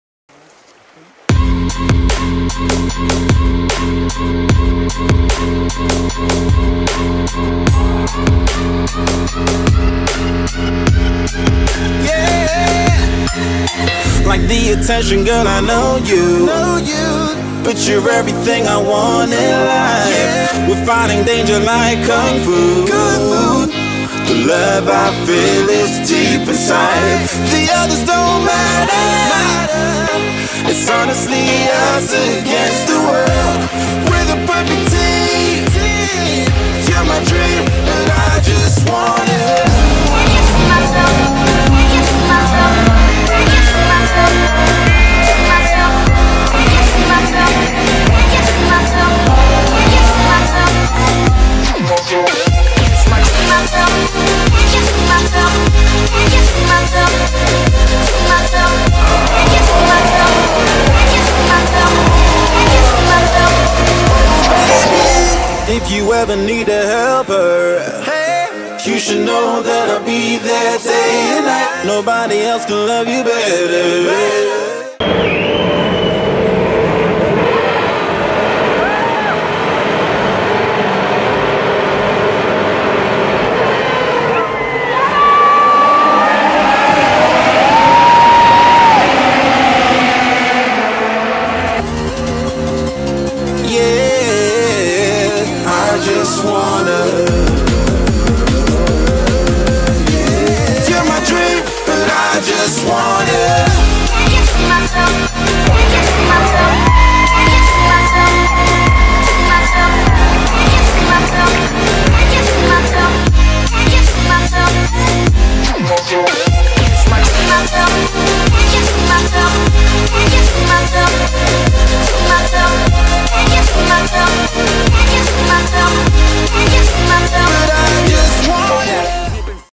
Postcard from Hungarian Grand Prix 2016 !
CHANGE TO HD!
Come with us for quick adventure in Hungaroring on F1 Hungarian Grand Prix 2016!

ALSO!